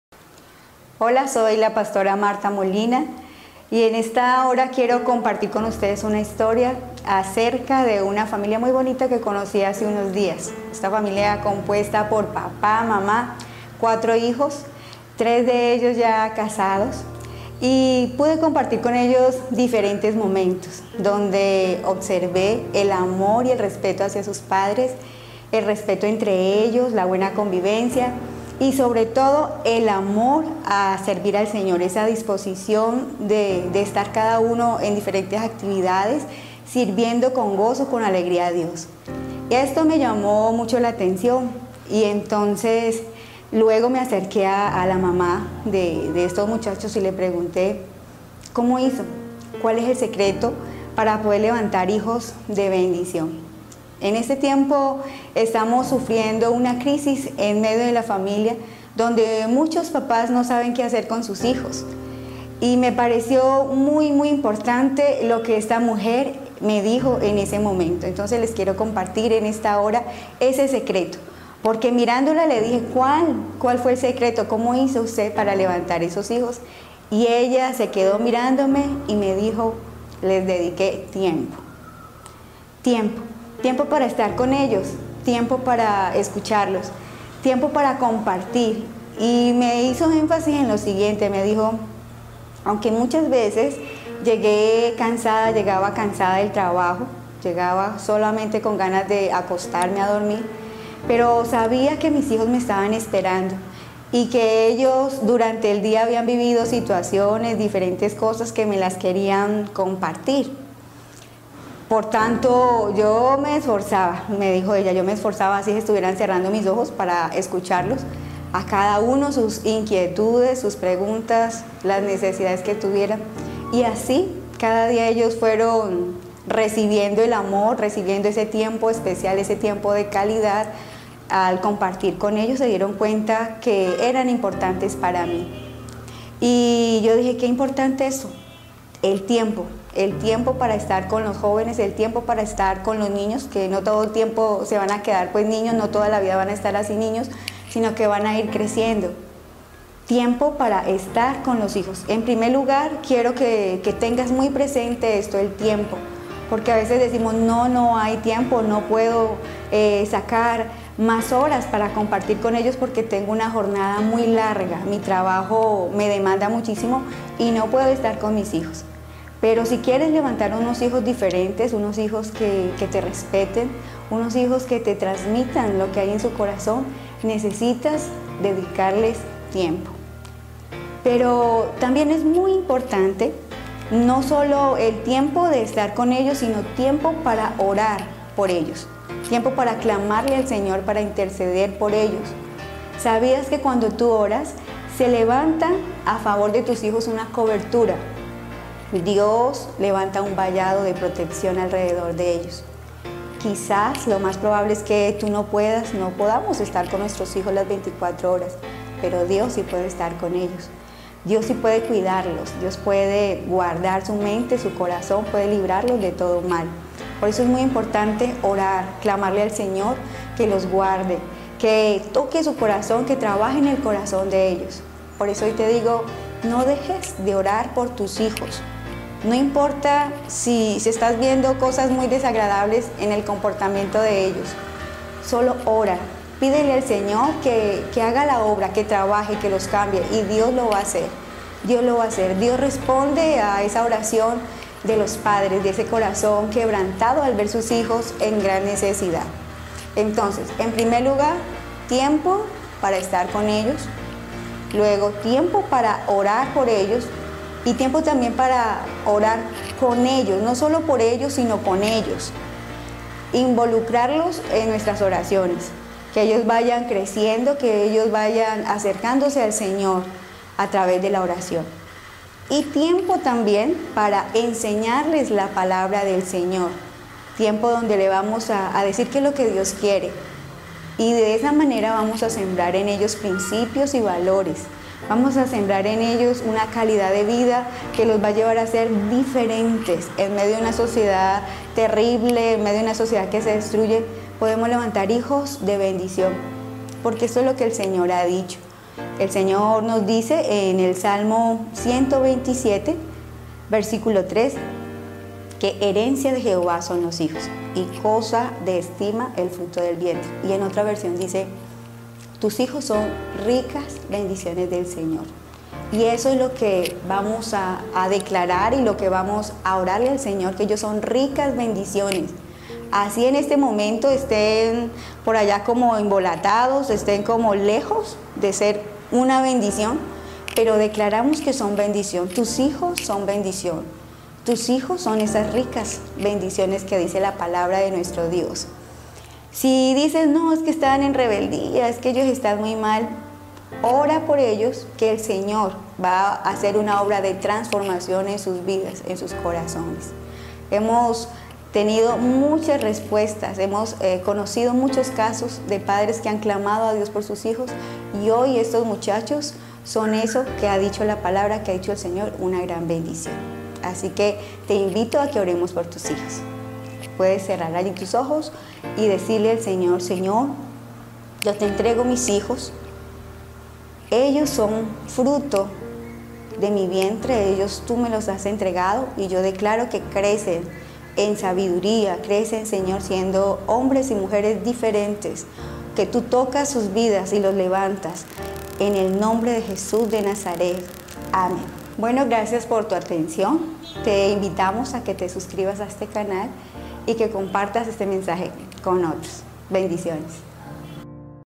¿CÓMO LEVANTAR HIJOS DE BENDICIÓN? / PASTORA MARTHA MOLINA
En este vídeo vas a encontrar  4 claves para  levantar hijos de bendicen  aplícalas y compártelas con otros padres Dios te bendiga.
COMPARTE CON OTROS NUESTROS VÍDEOS
 CRA 4 norte # 72 B 35 Barrio Guaduales (Cali - Colombia)